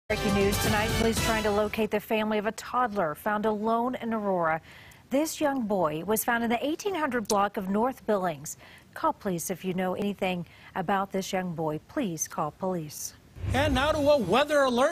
Aurora Police Find Toddler, Ask For Public's Help Locating Parents
Police in Aurora are searching for the parents or guardian of a toddler who was found on Thursday.